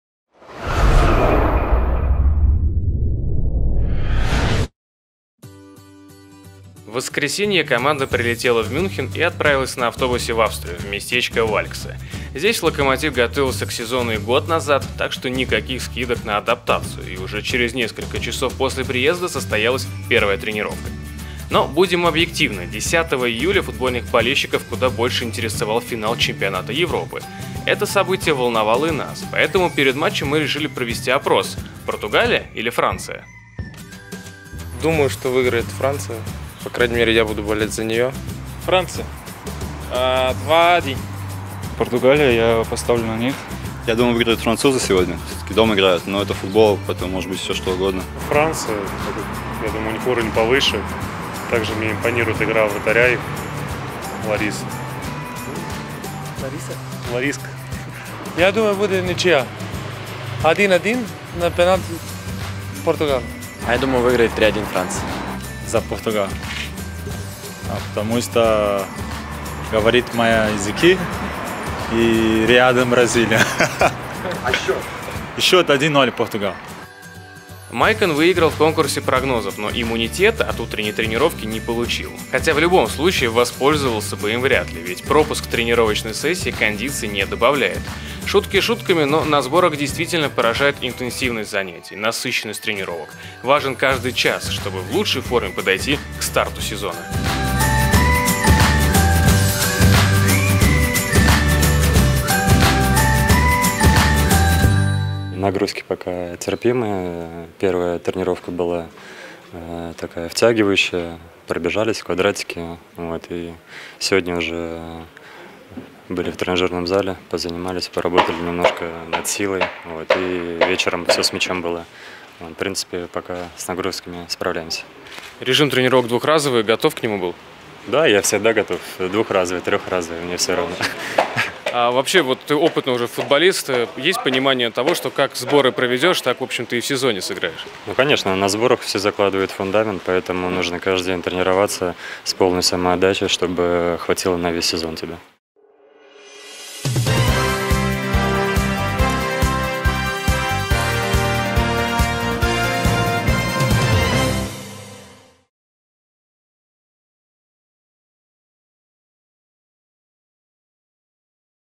«Нагрузки терпимые»
В воскресенье начался второй предсезонный сбор «Локомотива». Команда прилетела в Мюнхен, откуда на автобусе отправилась в австрийский Вальксе и в этот же день провела первую тренировку.